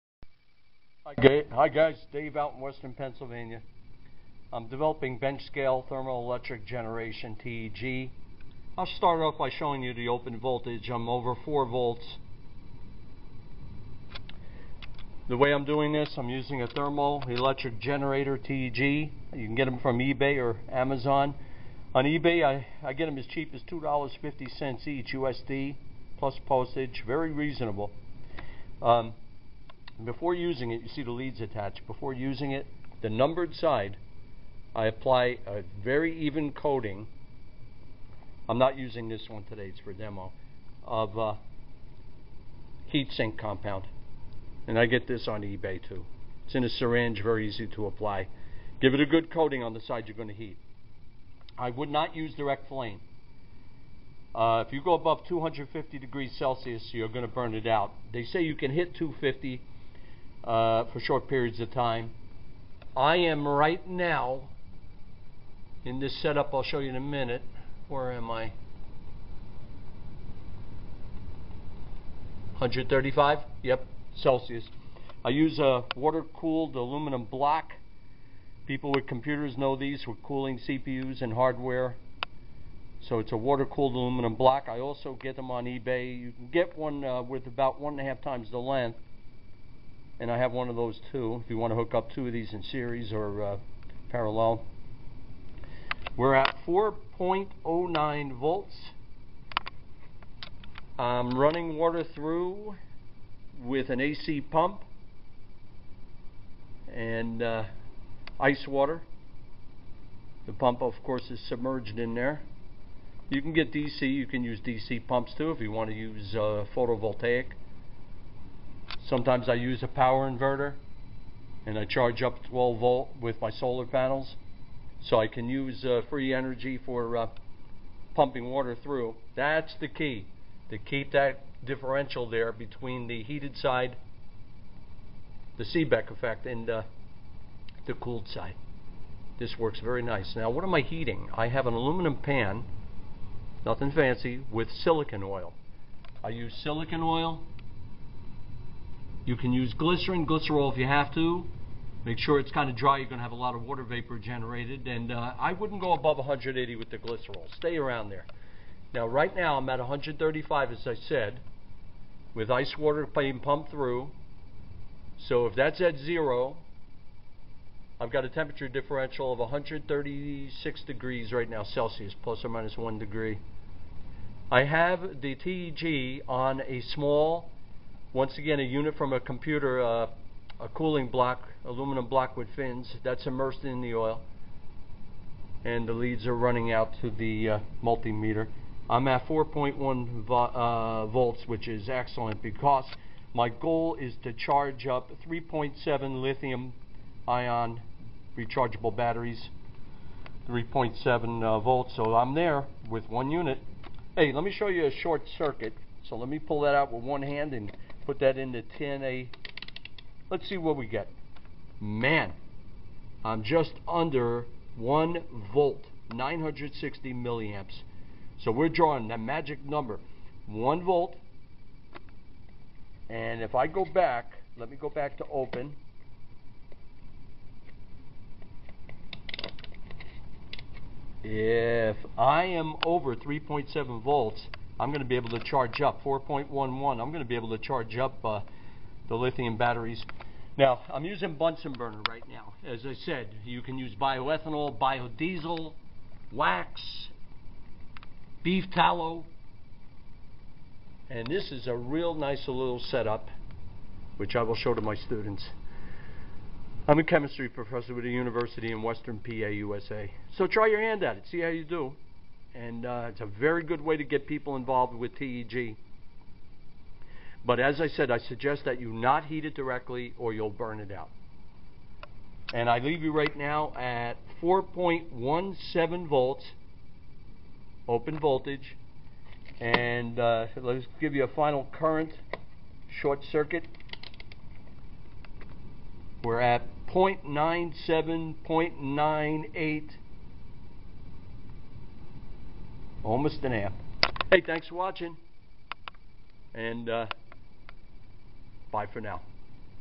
Thermoelectric Generation/ 4.25V/ 1.01 A/ 140 Deg. C
Bench-scale setup with a thermoelectric generator . The TEG is on an aluminum finned cooling block immersed in silicon oil. A water-cooled aluminum block is used on the lower temp. side and the ice water is pumped through with an AC pump ( you can use a DC pump/ solar-charged battery , of course). Here are some numbers: 4.25V (open)/ 1.01 A (short)/ 140 deg C temp. differential. I plan on charging 3.7V lithium ion batteries with this setup and will upload my progress to YouTube as I go along.